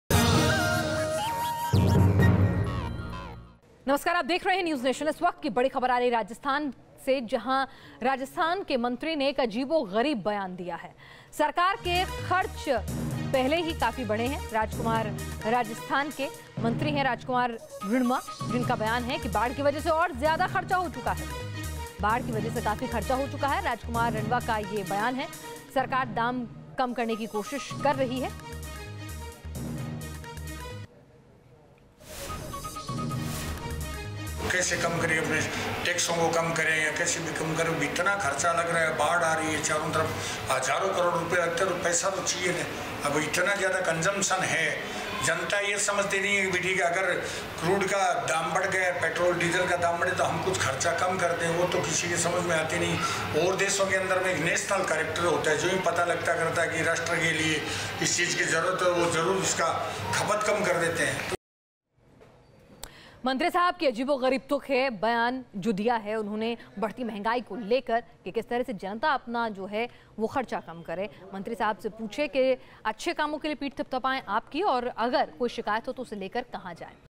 Fuel price hike: Rajasthan minister asks people to 'reduce expenses'
In the wake of the continuous hike in fuel prices, state minister and MLA from Rajasthan's Churu district, Rajkumar Rinwa, has appealed to people to reduce their expenses.